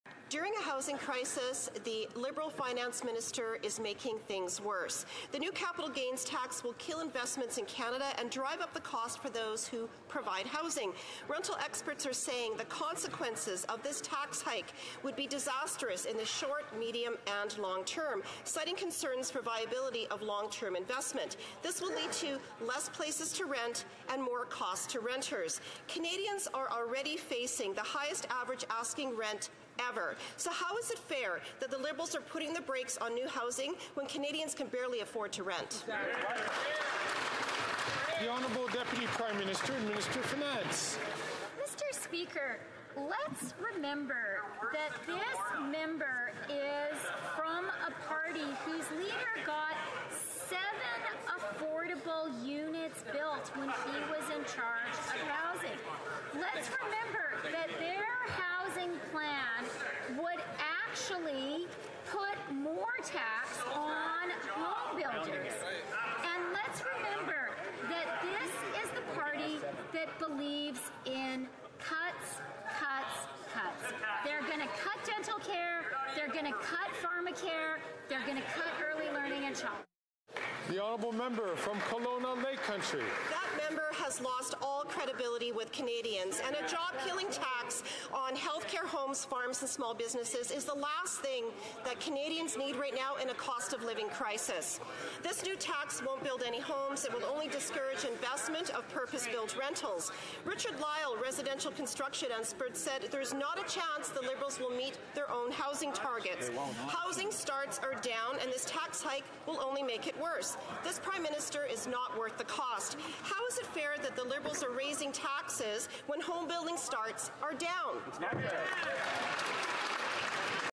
Canadians Don't Need More Taxes During the Cost of Living Crisis
Increasing taxes on healthcare, homes, farms, and small businesses is the last thing Canadians need during the cost of living crisis.